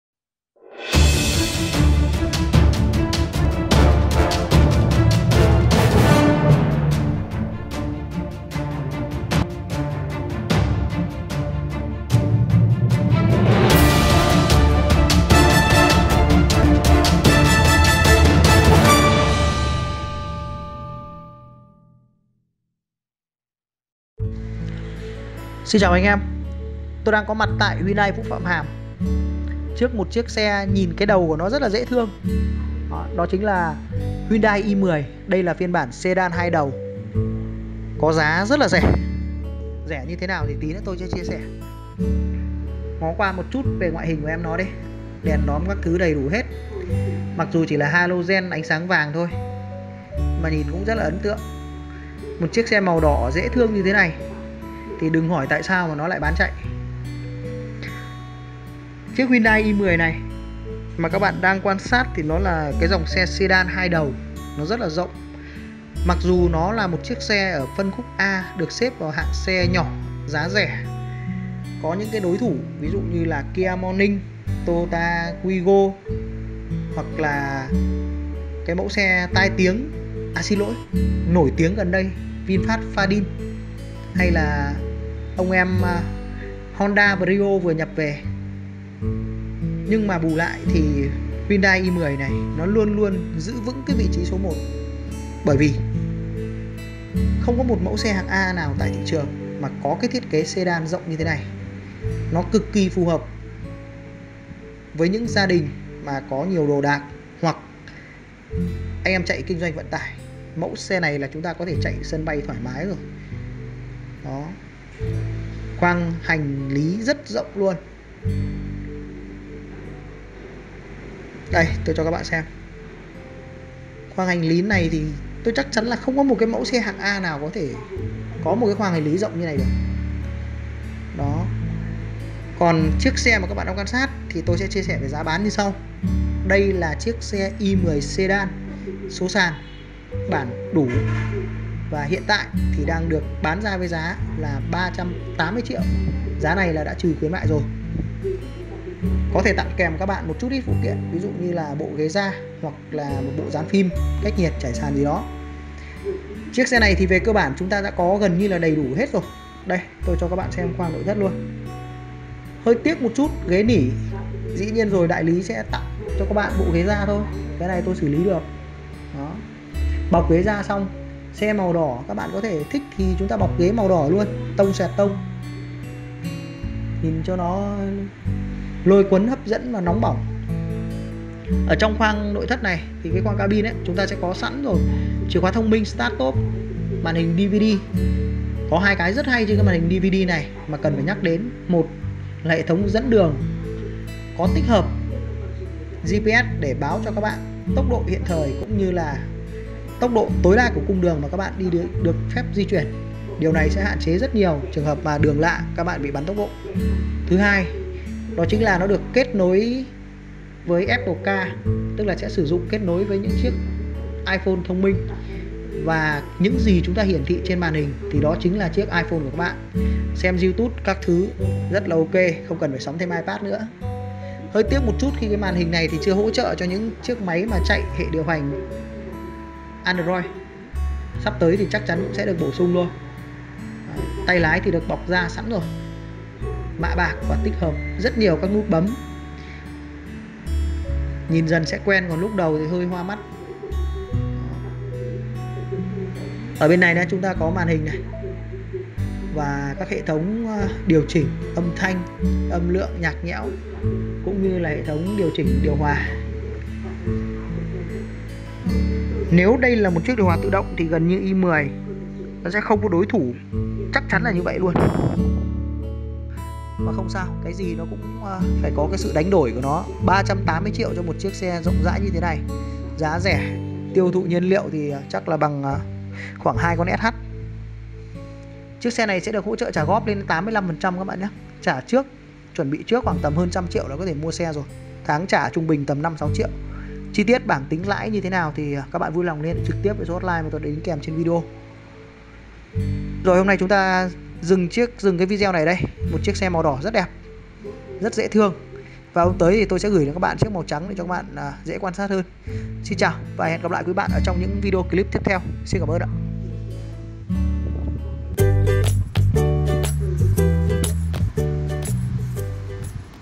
i10 Sedan 2020, đừng hỏi lý do vì sao bán chạy. NGON BỔ RẺ là có thật
Hyundai Grand i10 với 2 kiểu dáng sedan hoặc hatchback.